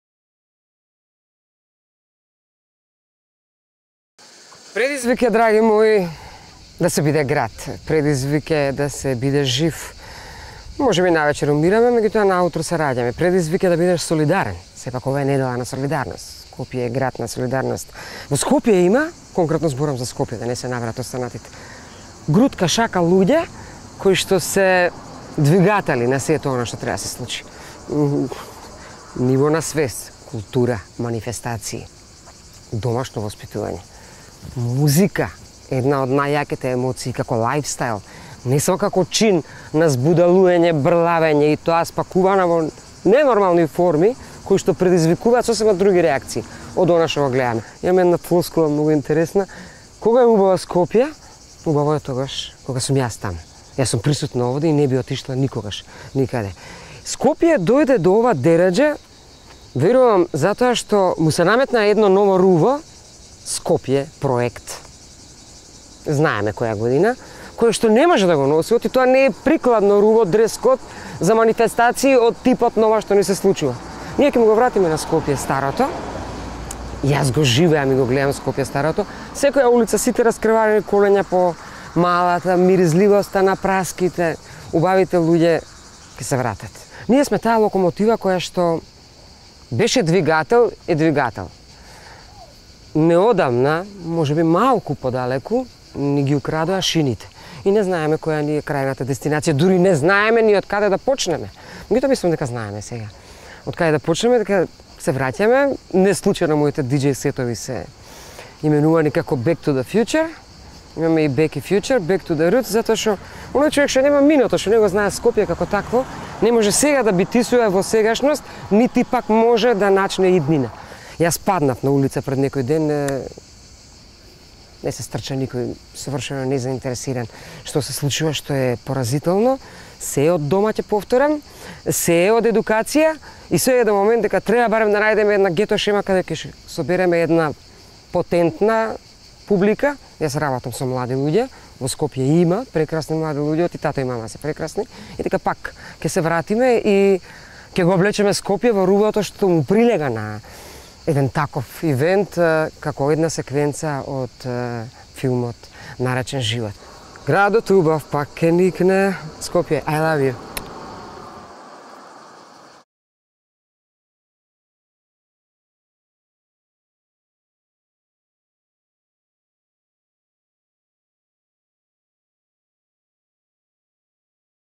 Вера Јанковиќ: Предизвик е да се биде град
Проектот "Скопје 2014" е неприкладно руво за нашиот главен град и верувам дека ќе го преоблечеме со соодветна музика, култура, манифестации, вели водителката и ди-џеј Вера Јанковиќ.  Градот на солидарноста ја изгуби емпатијата, во пресрет на одбележувањето на годишнината од земјотресот во Скопје во 1963 година.